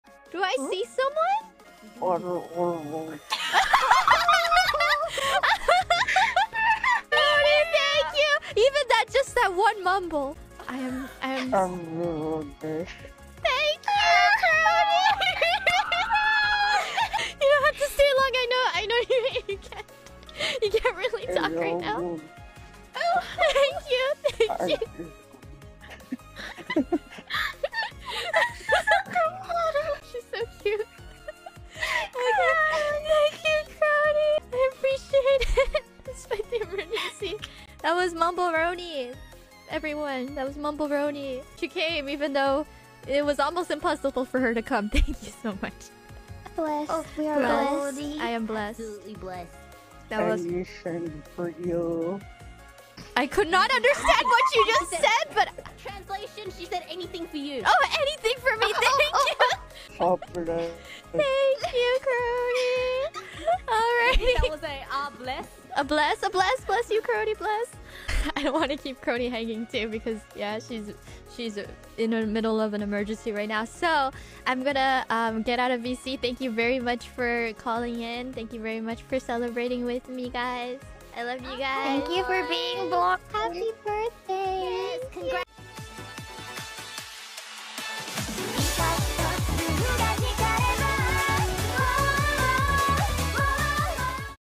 𝐊𝐫𝐨𝐧𝐢𝐢 𝐜𝐚𝐥𝐥'𝐬 𝐢𝐧 𝐈𝐑𝐲𝐒 𝐛𝐢𝐫𝐭𝐡𝐝𝐚𝐲 𝐰𝐡𝐢𝐥𝐞 𝐢𝐧 𝐭𝐡𝐞 𝐦𝐢𝐝𝐝𝐥𝐞 𝐨𝐟 𝐄𝐦𝐞𝐫𝐠𝐞𝐧𝐜𝐲 |Hololive EN
『HopeyBirthday』What a SPECIAL DAY!!🎈🎂【2023 BIRTHDAY STREAM】

Source:
IRyS Ch. Hololive EN -
Ouro Kronii Ch. Hololive EN -

ED ART by @Tkminnm from Twitter -

BGM used
DOVA-SYNDROME